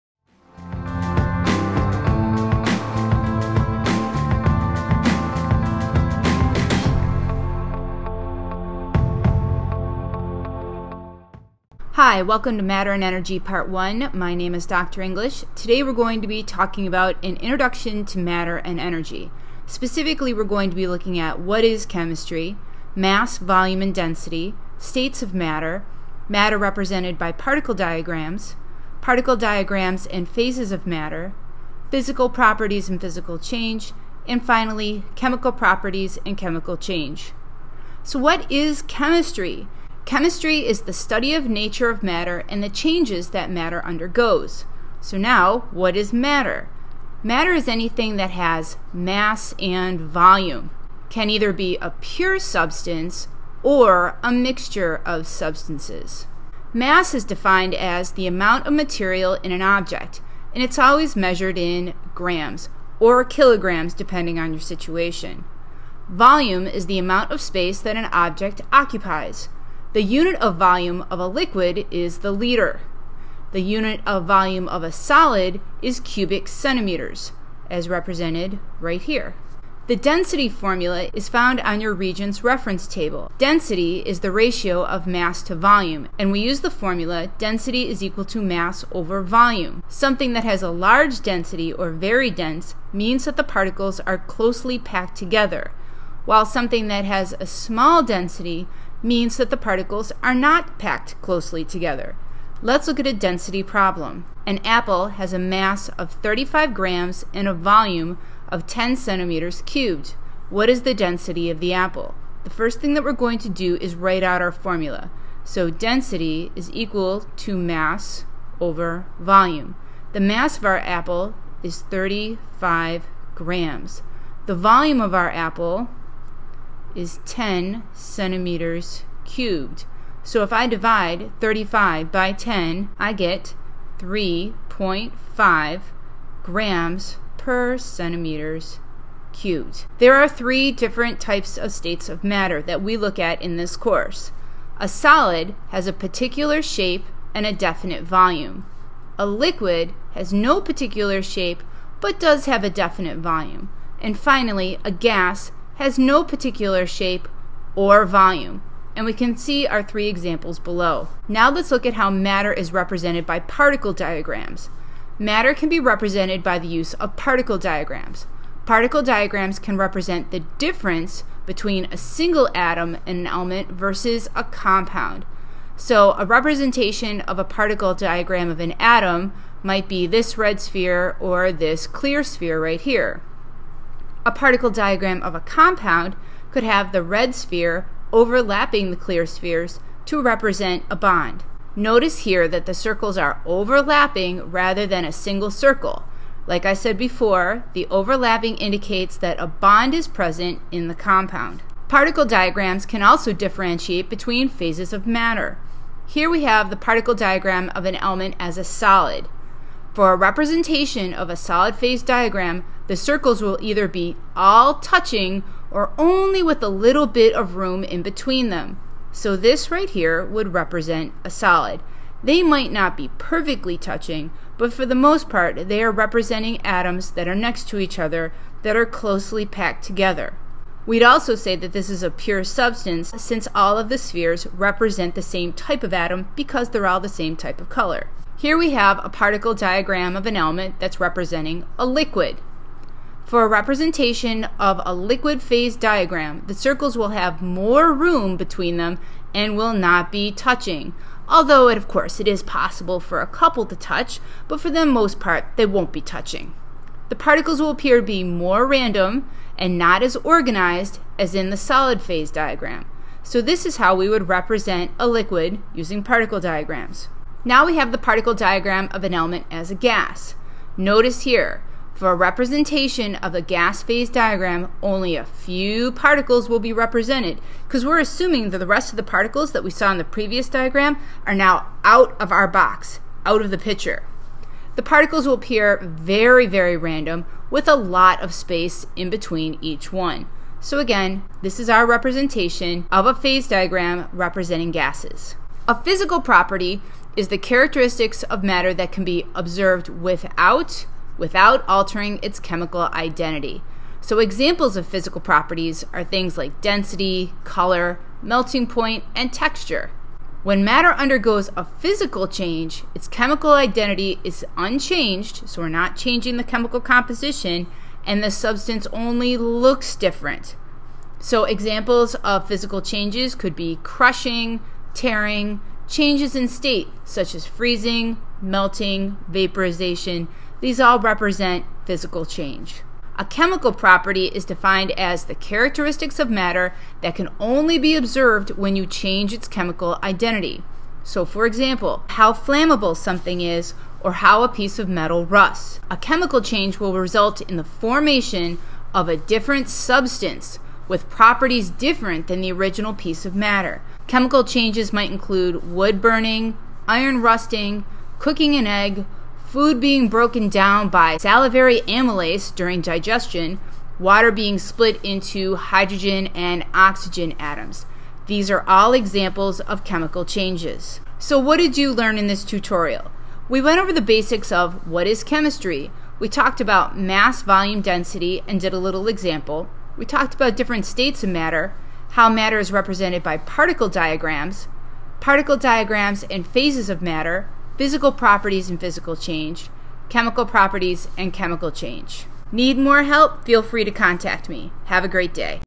Matter and Energy Part 1 Introduction to Matter and Energy
This video provides a very general introduction to the concepts of matter and energy. The concept of matter is defined along with mass, volume and density. Physical properties and change is contrasted with chemical properties and change.